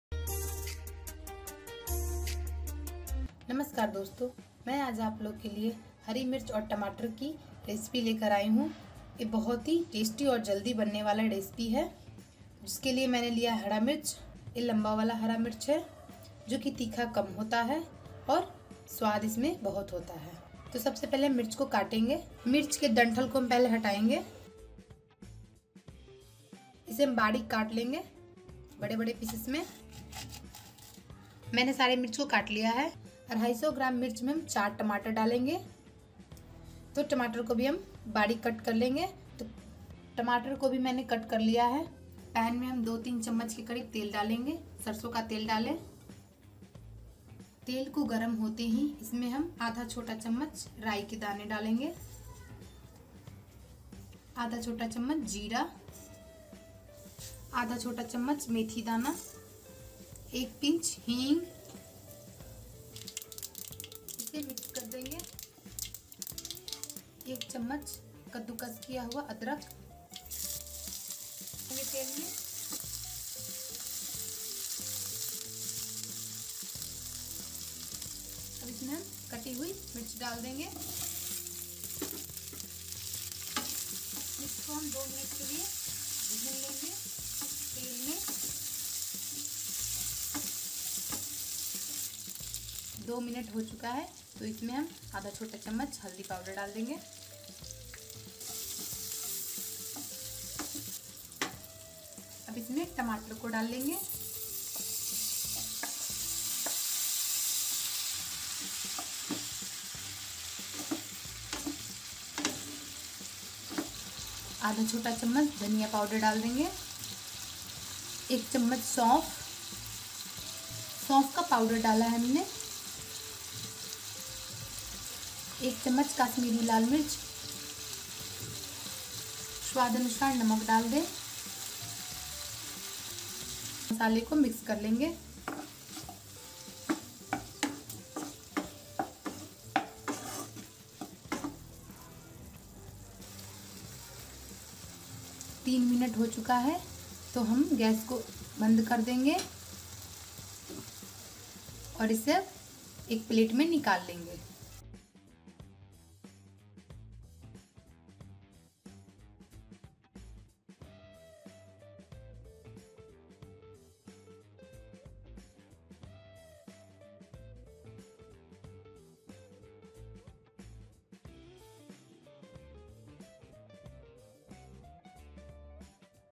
Hari mirch ki sabji kaise banaye हरी मिर्च की सब्जी ऐसे बनाओगे तो हर कोई तारीफ करेगा Tomato Kitchen
Hari mirch ki sabji kaise banaye,  Mirchi ki sabji / Mirchi ka salan. Hello friends today i am going to share recipe of hare mirch ki sabji. This recipe is very easy to cook and taste delicious.